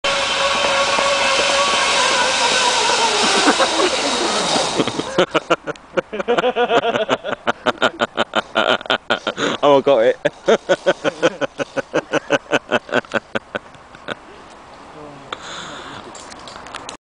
Go Ape Zip Landing (Me)
My amazing backwards zip landing filmed by Leigh